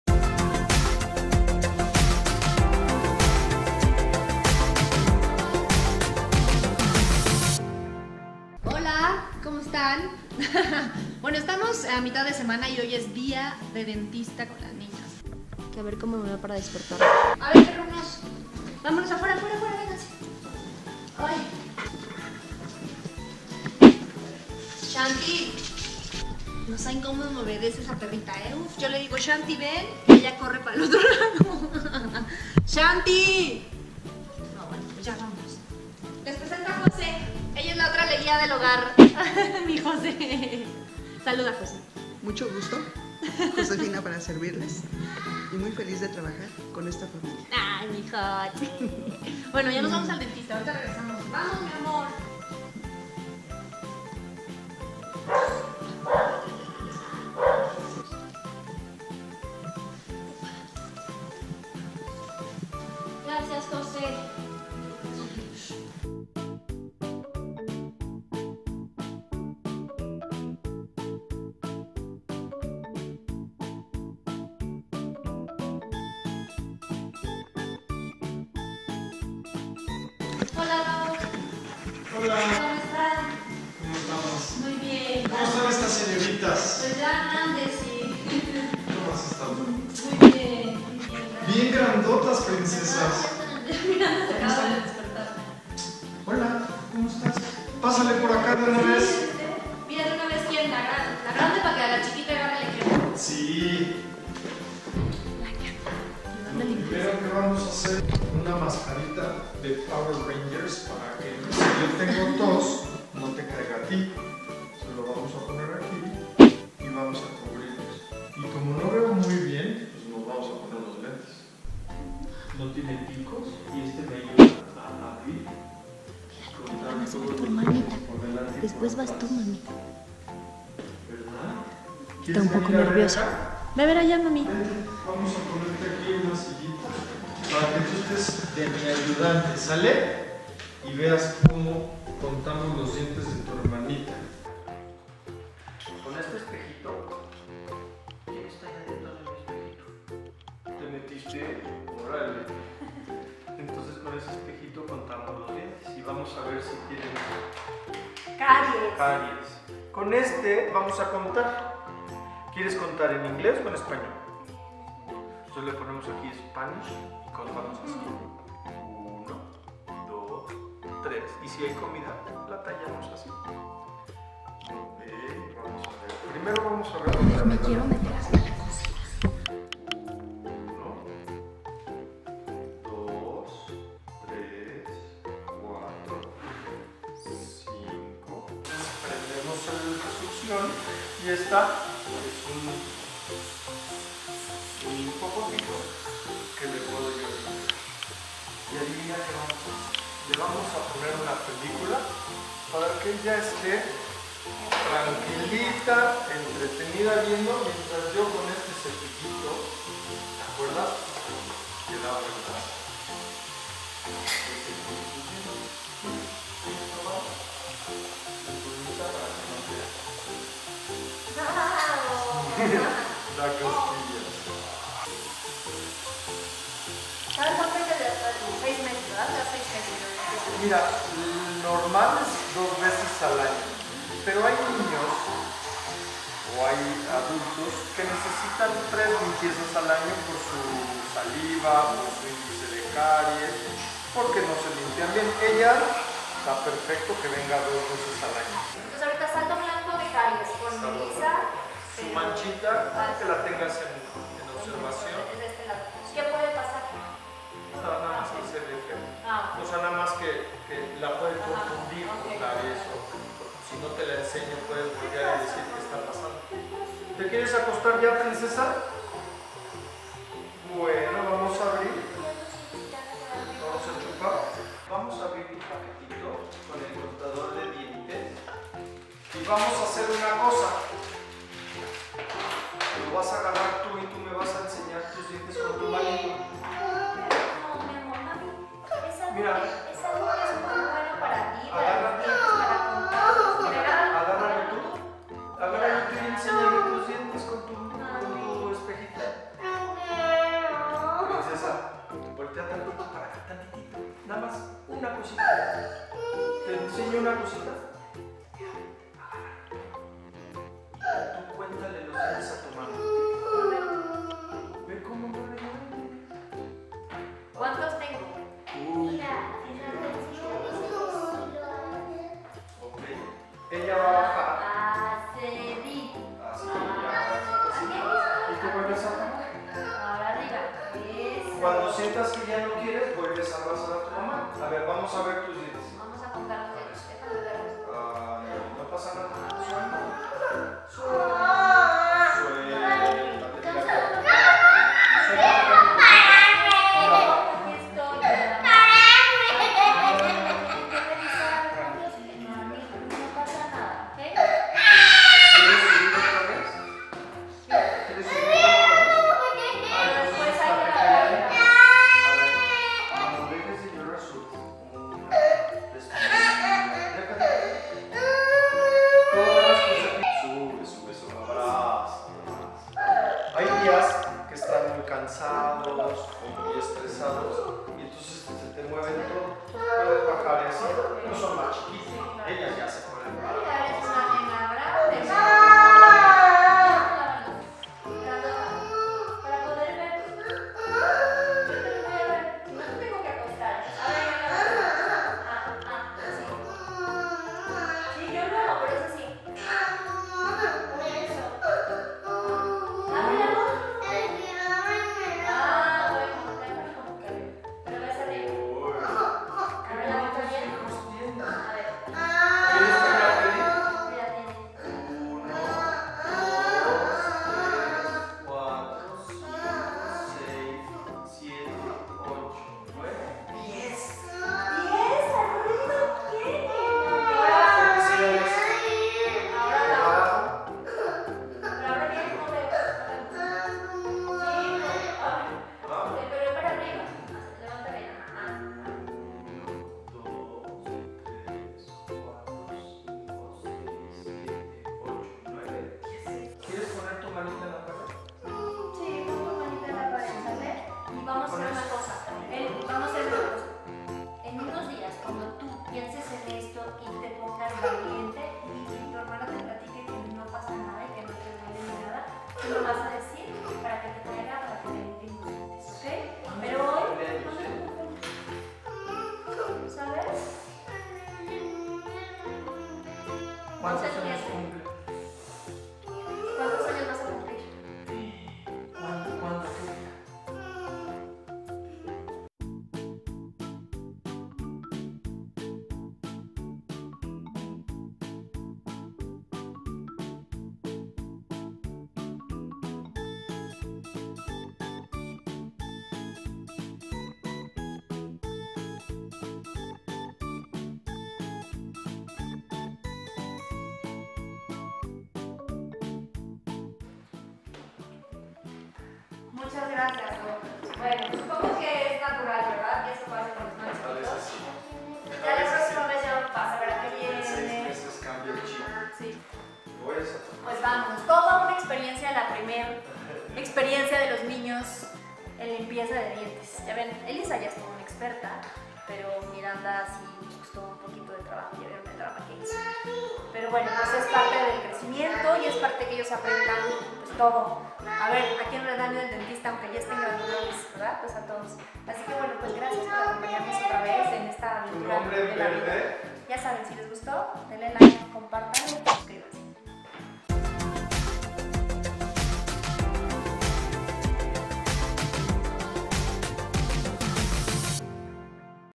Día de dentista con las niñas :: Geraldine Bazán
No saben todo lo que pasó en el primer día con el dentista para Miranda.